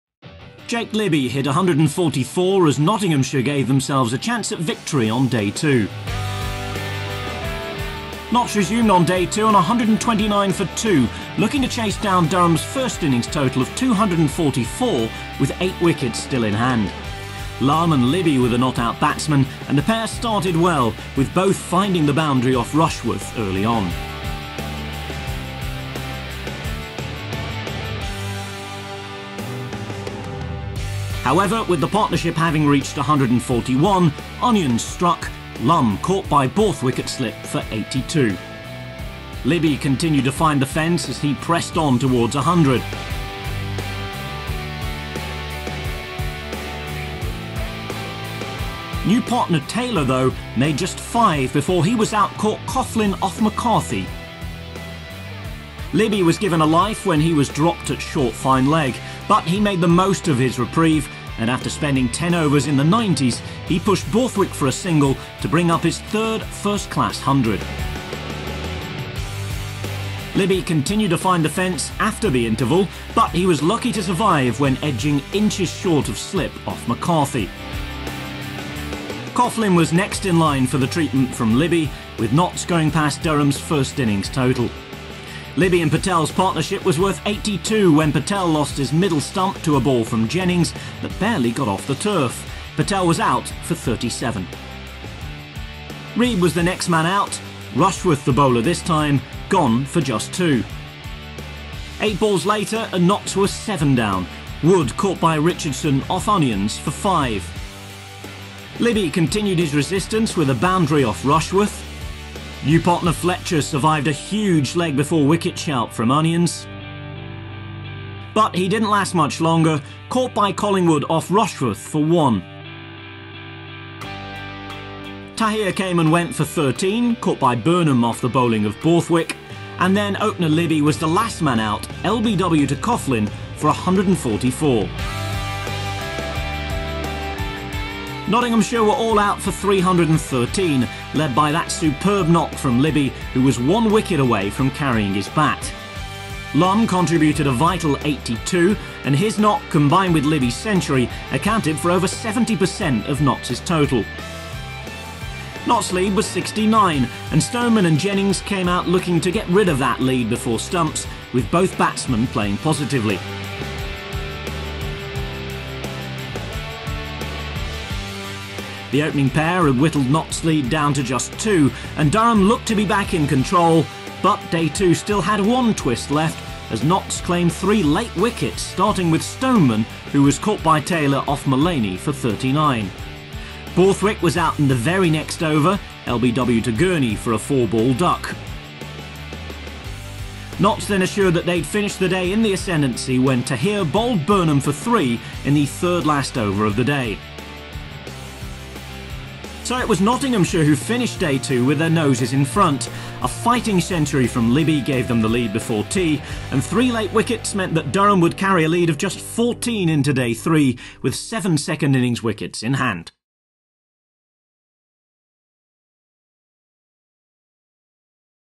Libby hundred puts Notts in ascendancy, Durham v Notts, Day Two
Jake Libby went close to carrying his bat for Nottinghamshire in their Specsavers County Championship match against Durham at Chester-le-Street.